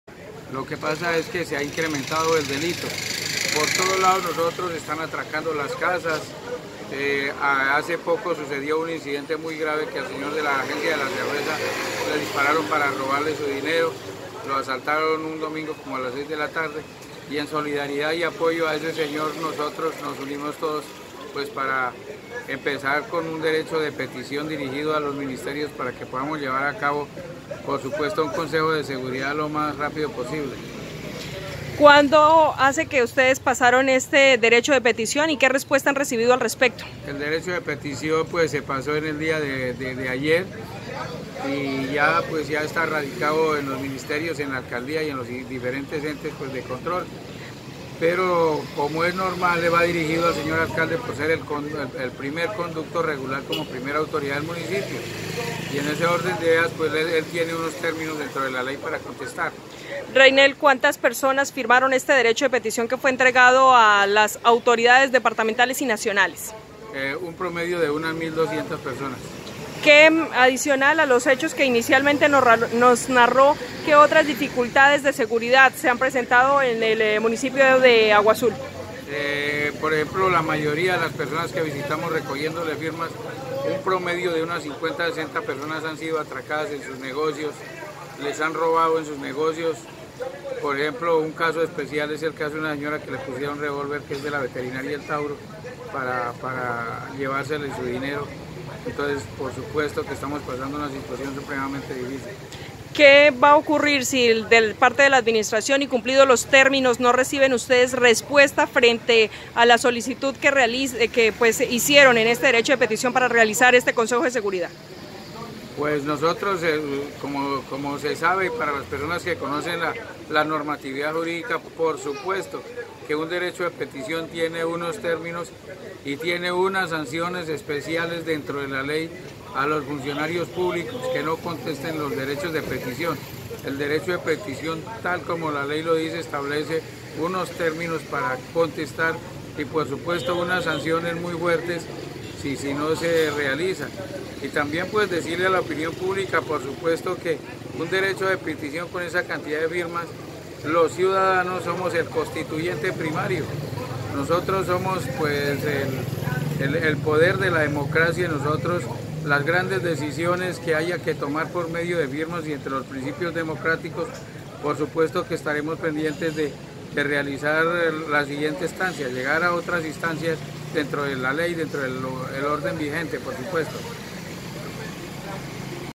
Por derecho de petición comunidad y comerciantes de Aguazul, exigen consejo de seguridad
Cansados de los constantes atracos, hurtos y la inseguridad incluso hasta para desplazarse de barrios alejados hasta la zona céntrica del municipio, después de las 7 de la noche, más de mil habitantes de esta localidad apoyaron con su firma la exigencia de un consejo de seguridad con presencia incluso del Ministerio del Defensa, al alcalde Renier Darío Pachón.
José Reinel Larrota, representante del  comercio informal del municipio de Aguazul, asegura que ya no soportan más hechos de inseguridad, pues están siendo objetos de atracos en los establecimientos comerciales, incluso a plena luz del día, sin que las autoridades tomen medidas o estrategias que generen algún tipo de impacto en esta problemática.
El derecho de petición fue radicado ante la alcaldía de Aguazul el día martes, y según los firmantes esperaran a que se cumplan los términos de ley para respuesta de parte de la administración municipal, o de lo contrario aplicarán las medidas legales determinadas para el silencio administrativo en caso de no recibir respuestas.